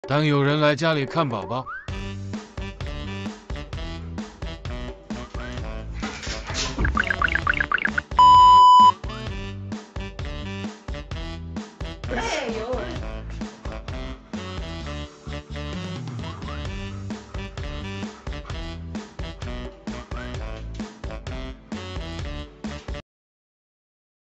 Adorable Alaskan Malamute Welcomes Visitors to Meet the Baby! Watch Him Be the Cutest...
In this wonderful video, witness the undeniably cute Alaskan Malamute as he welcomes visitors to meet the baby. Get ready to be overwhelmed by the sheer adorableness of this furry friend! 🐾💕